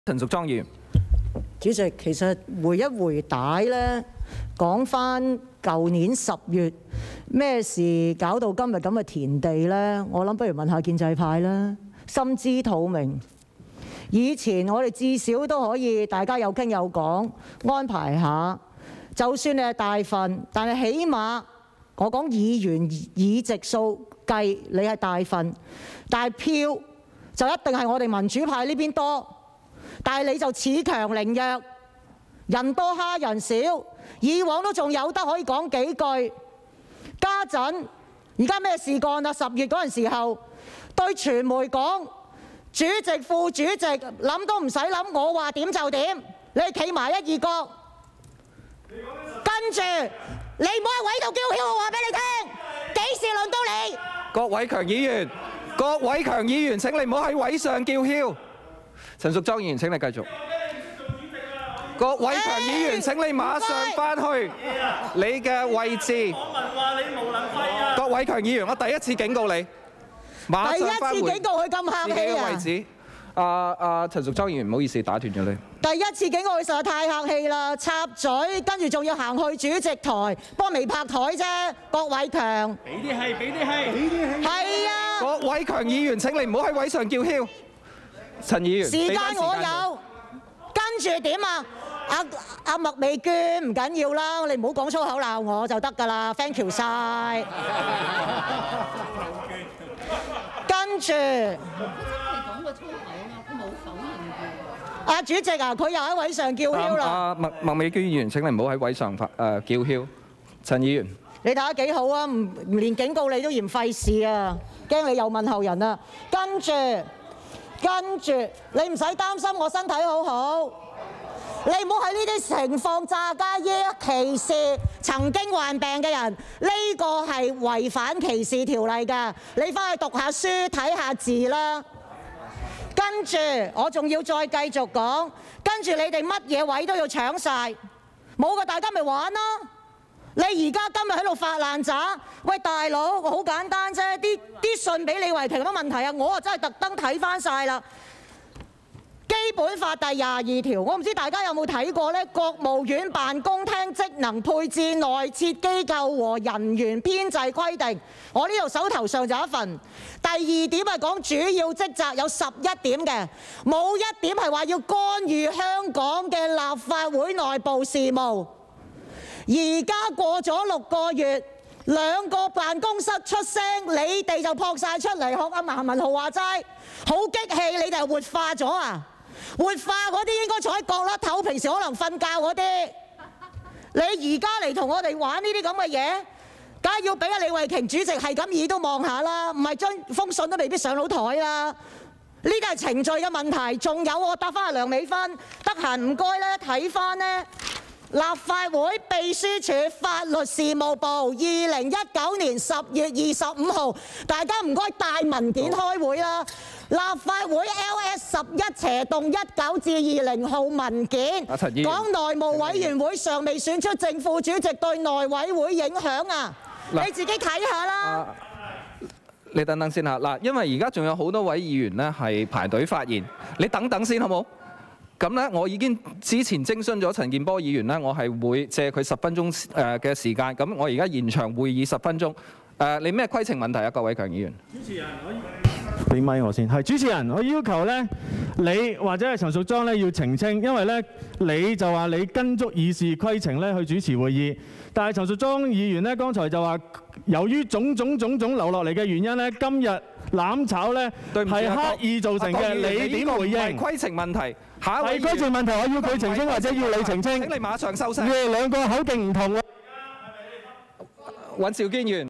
陳淑莊：麥美娟，唔好用粗口鬧我，就得㗎喇。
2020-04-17 內務委員會特別會議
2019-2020年度會期內務委員會主席及副主席的選舉